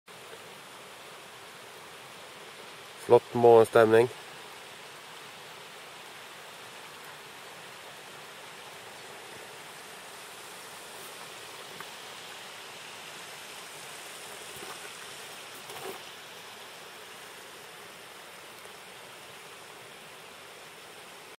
Morning scenery Salmonfishing in Gaula
Morning scenery Salmonfishing in Gaula. I got a nice 8kg salmon on the flie just before this video was taken, I tend to get most fish in the hours between 23 and 05 for some reason.